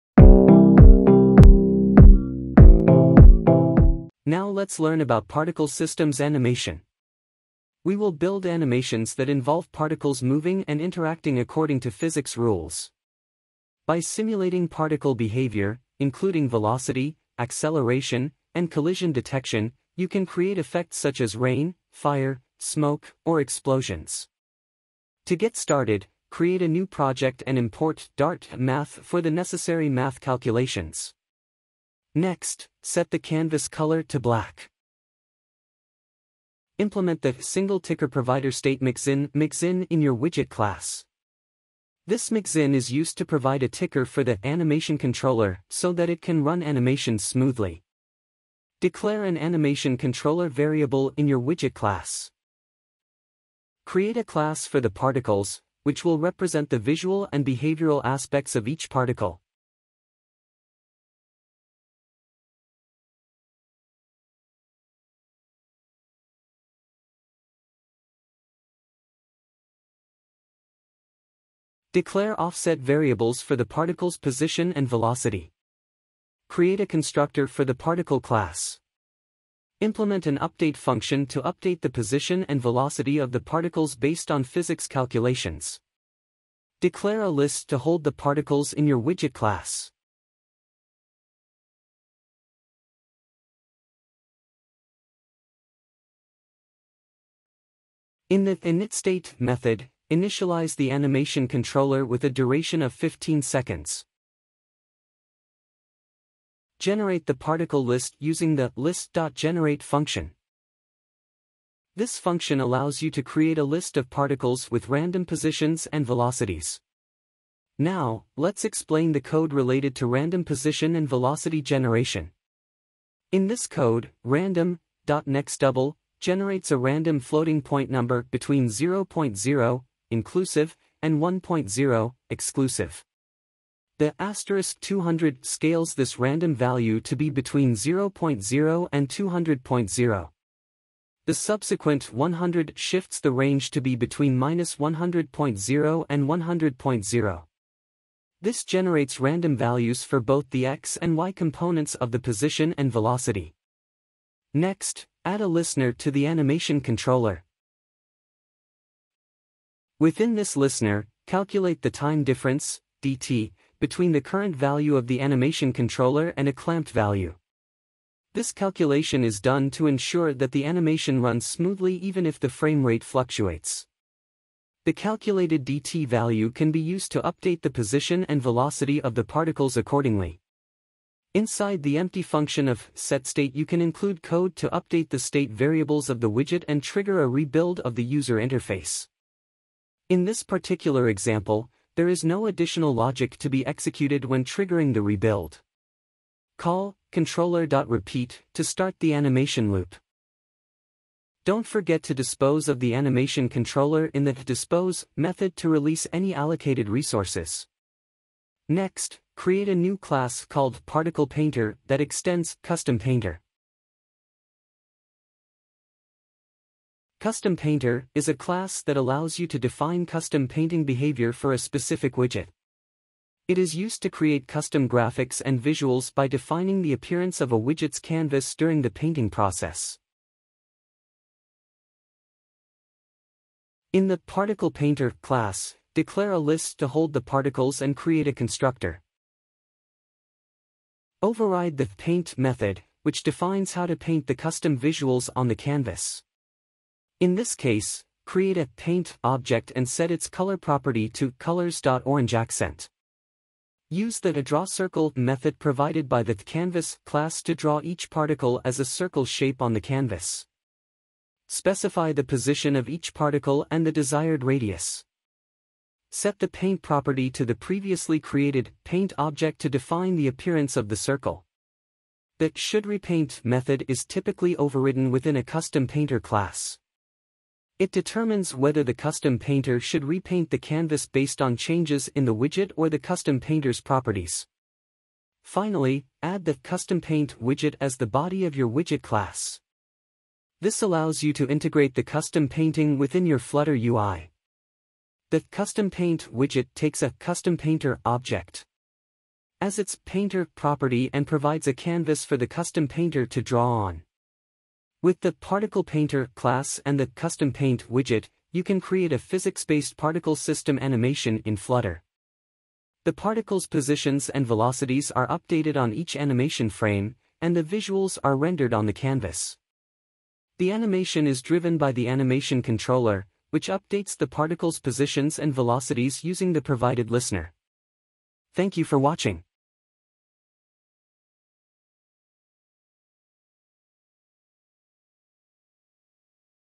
FLUTTER ANIMATION - 15 | Particle Systems | PHYSICS-BASED ANIMATION
Build animations involving particles that move and interact according to physics rules. You can create effects such as rain, fire, smoke, or explosions by simulating particle behavior, including velocity, acceleration, and collision detection.

we create a physics-based particle system animation in Flutter. The particles are represented by the `Particle` class, which has a position and velocity. The `update` method of the `Particle` class is called on each animation frame to update the position and velocity of the particles based on physics calculations. The `ParticlePainter` class is responsible for rendering the particles on the canvas using `CustomPainter`. The animation is driven by an `AnimationController`, which updates the particles' positions and velocities using the `addListener` callback.